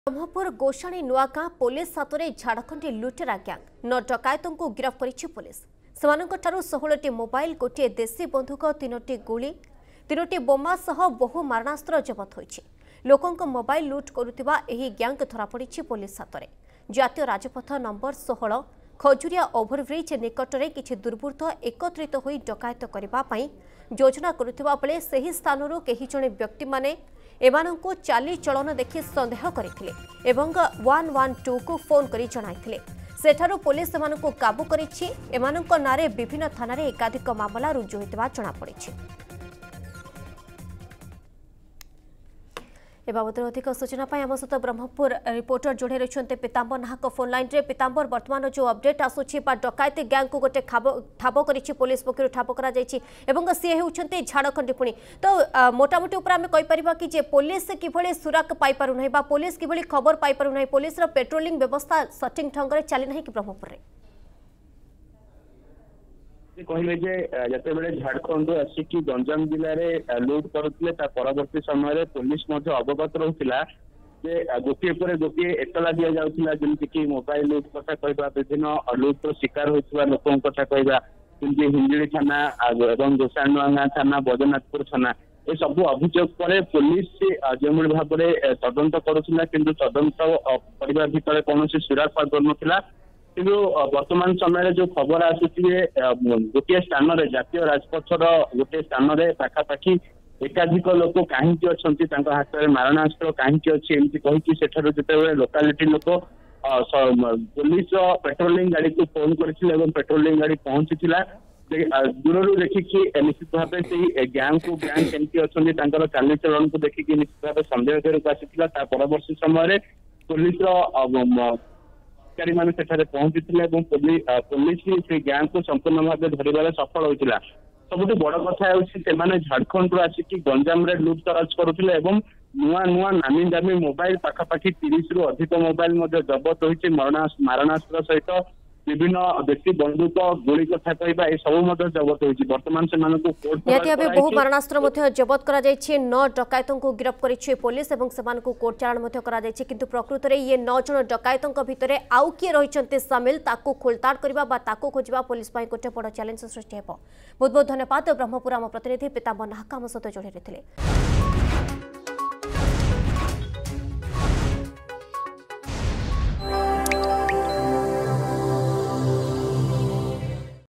ବ୍ରହ୍ମପୁର ଗୋଷାଣୀନୂଆଗାଁ ପୋଲିସ ହାତରେ ଝାଡ଼ଖଣ୍ଡୀ ଲୁଟେରା ଗ୍ୟାଙ୍ଗ
ବ୍ରହ୍ମପୁର ଗୋଷାଣୀନୂଆଗାଁ ପୋଲିସ ହାତରେ ଝାଡ଼ଖଣ୍ଡୀ ଲୁଟେରା ଗ୍ୟାଙ୍ଗ । 9 ଡକାୟତଙ୍କୁ ଗିରଫ କରିଛି ପୋଲିସ । ସେମାନଙ୍କ ଠାରୁ 16ଟି ମୋବାଇଲ, ଗୋଟିଏ ଦେଶୀ ବନ୍ଧୁକ, 3 ଗୁଳି, 3ଟି ବୋମା ସହ ବହୁ ମାରଣାସ୍ତ୍ର ଜବତ ହୋଇଛି ।